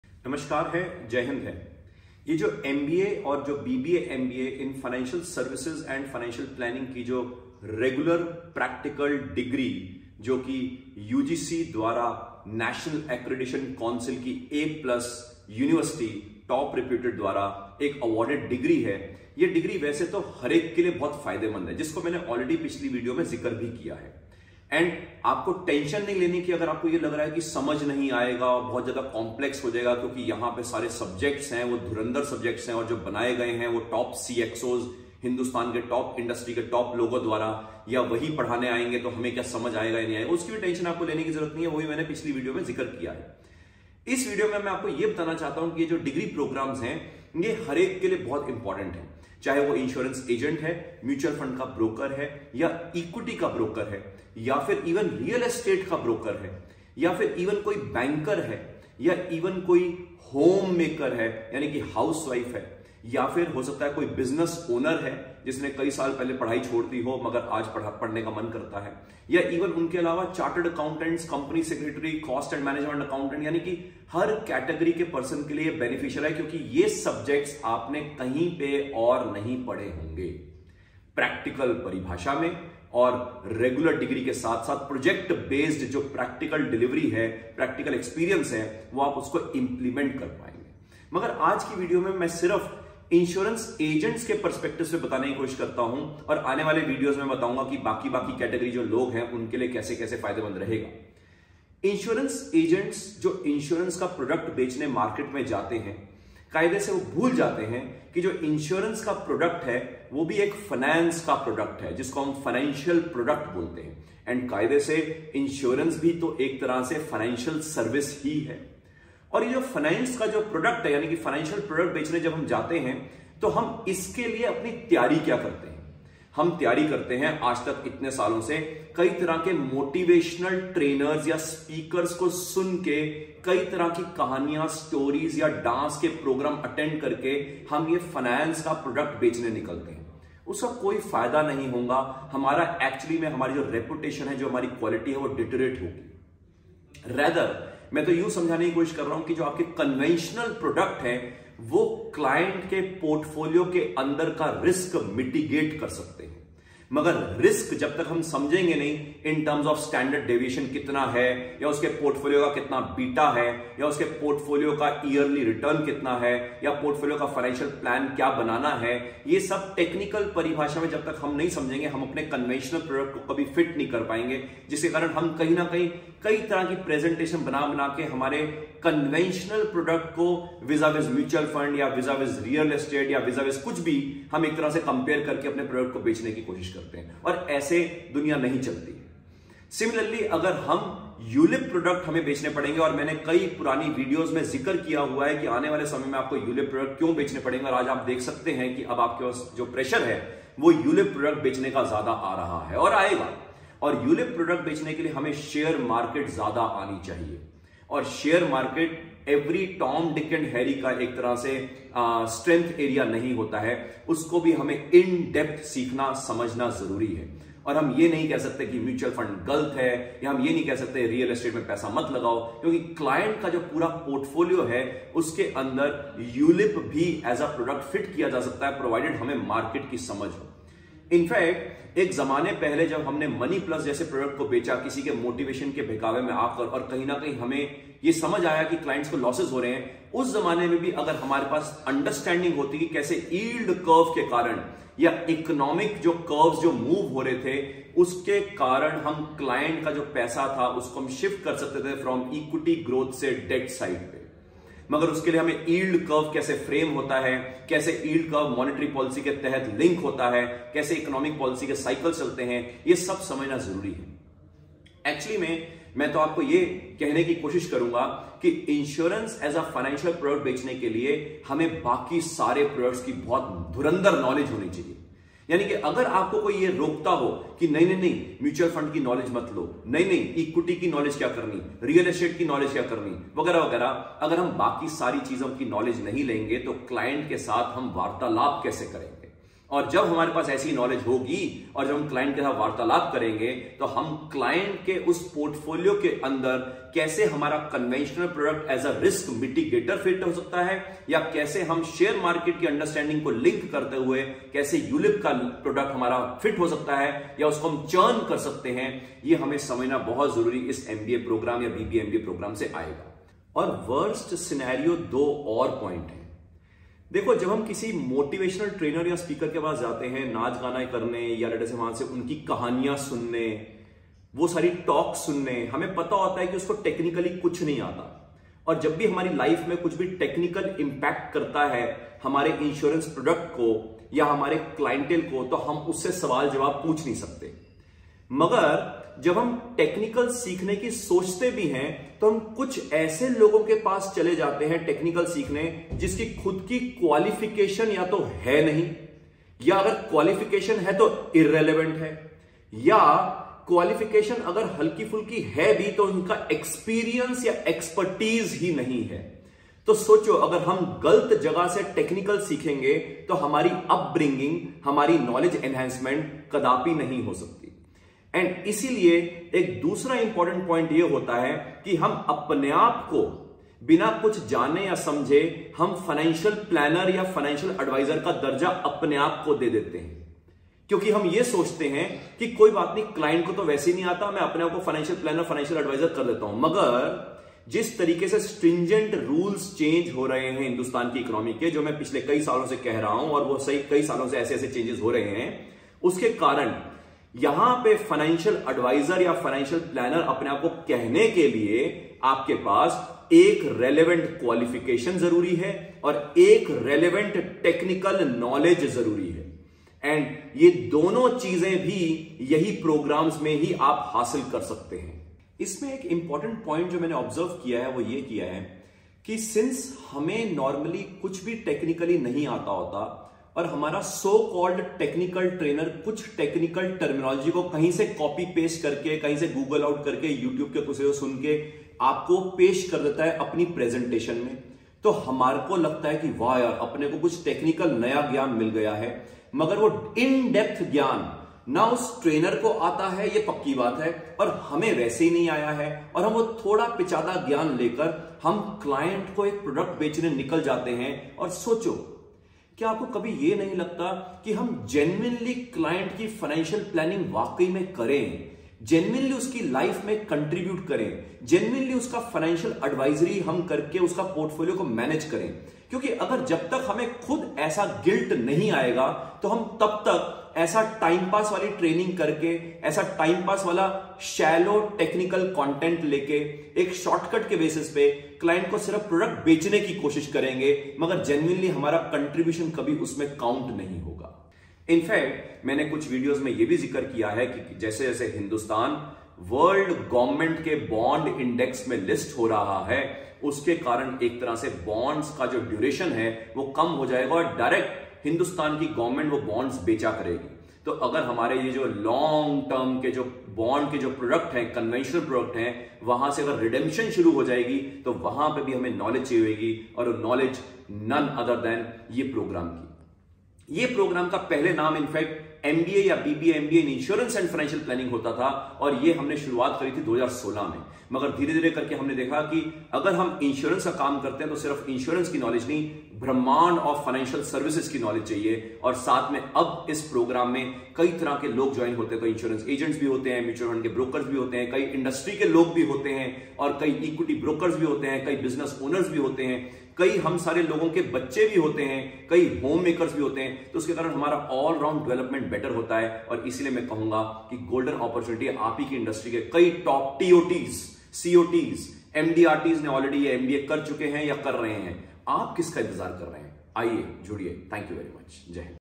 Importance of Degree in Financial Services & Financial Planning for Insurance Agents | Chitkara Univ
CA Dr Aman Chugh explains about the Importance of learning and pursuing these Degree Programs in Financial Services & Financial Planning at Chitkara University especially for Insurance Agents.

To know more about BBA-MBA/MBA in Financial Services & Financial Planning, please visit bfsi.chitkara.edu.in or call at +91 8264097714.